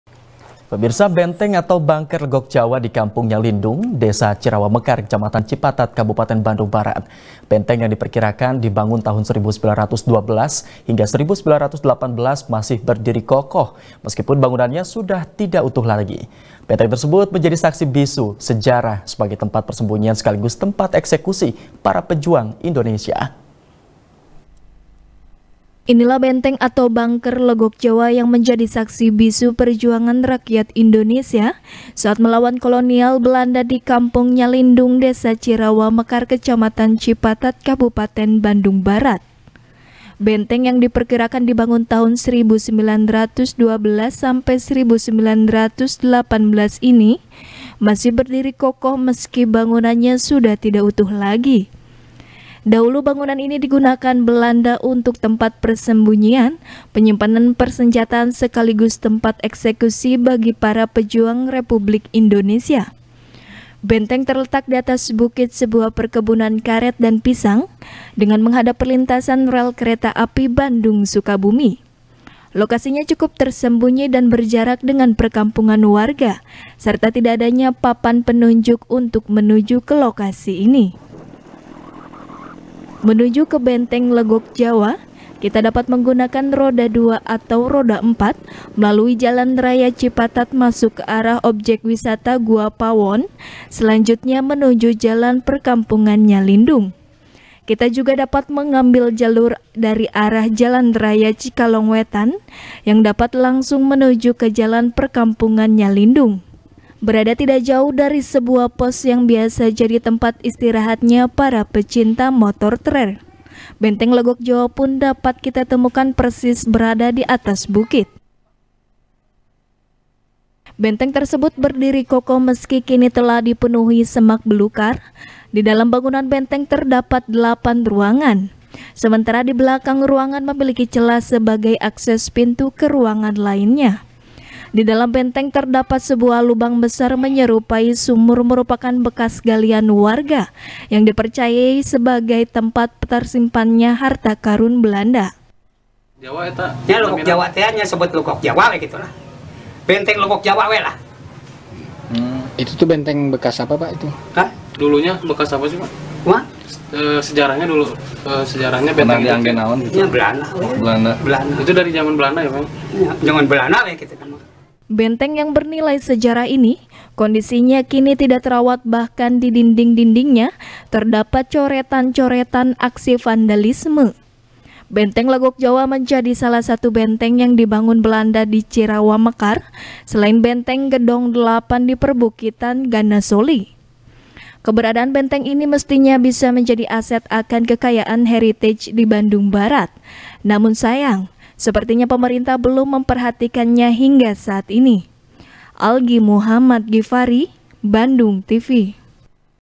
Benteng Legokjawa, Saksi Bisu Sejarah Peninggalan Belanda Yang Tak Terawat | SBR | BANDUNGTV
SBR | BANDUNGTV | RIYADI RIFKY | Selama puluhan tahun, sebuah dusun di Kabupaten Sumedang, Jawa Barat, terisolasi dari pusat desa maupun kota akibat tidak adanya akses jalan untuk kendaraan. Untuk membawa orang sakit saja, warga harus mengangkut pasien dengan berjalan kaki, karena tidak adanya akses kendaraan. Namun setelah 50 tahun terisolir, kini akses jalan Dusun Jeunjing telah terbuka dengan gotong royong antara warga bersama dengan TNI.